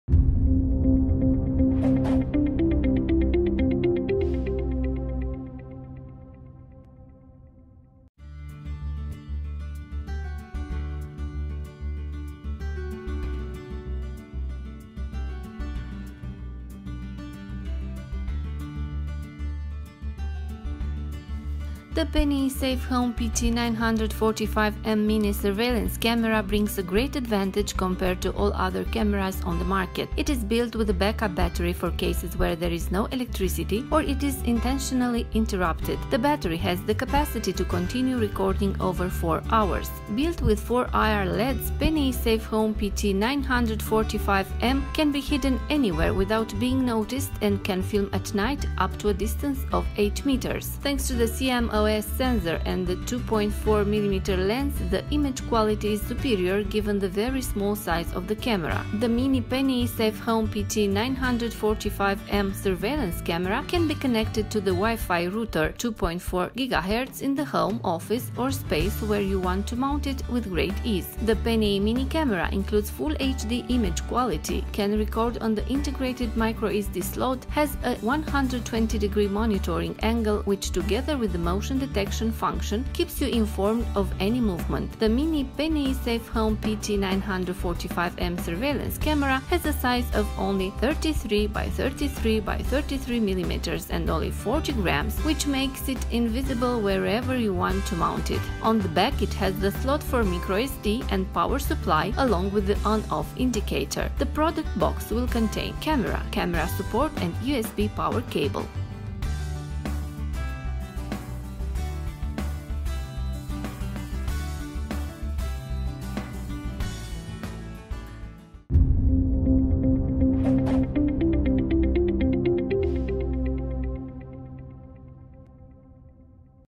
PNI SafeHome PT945M 1080P WiFi mini surveillance camera TR
PNI SafeHome PT945M mini gözetleme kamerası, piyasadaki diğer tüm kameralara kıyasla büyük bir avantaj sağlar. Elektriğin olmadığı veya isteyerek kesildiği durumlar için yedek akü ile üretilmiştir. Pil, 4 saatin üzerinde kayda devam etme kapasitesine sahiptir.
4 adet IR LED ile üretilen PNI SafeHome PT945M, fark edilmeden herhangi bir yere gizlenebilir ve geceleri 8 metre mesafeye kadar çekim yapabilir.
CMOS sensörü ve 2,4 mm lens sayesinde, kameranın çok küçük boyutu göz önüne alındığında görüntü kalitesi üstündür.
Mini PNI SafeHome PT945M gözetleme kamerası, evde, ofiste veya monte etmek istediğiniz alanda büyük kolaylıkla WiFi yönlendiriciye (2,4 GHz) bağlanabilir.
PNI mini kamera, Full HD görüntü kalitesi içerir, entegre microSD yuvasına kayıt yapabilir, 120 derecelik izleme açısına sahiptir ve hareket algılama işleviyle birlikte sizi her türlü hareketten haberdar eder.
Mini PNI SafeHome PT945M gözetleme kamerasının yalnızca 33 X 33 X 33 mm boyutu ve yalnızca 40 gram ağırlığı vardır, bu da onu monte etmek istediğiniz her yerde görünmez kılar.
Arkasında, ON/OFF göstergesi ile birlikte mikro SD ve güç kaynağı yuvası vardır.
Ürün kutusu şunları içerecektir: kamera, kamera desteği ve USB güç kablosu.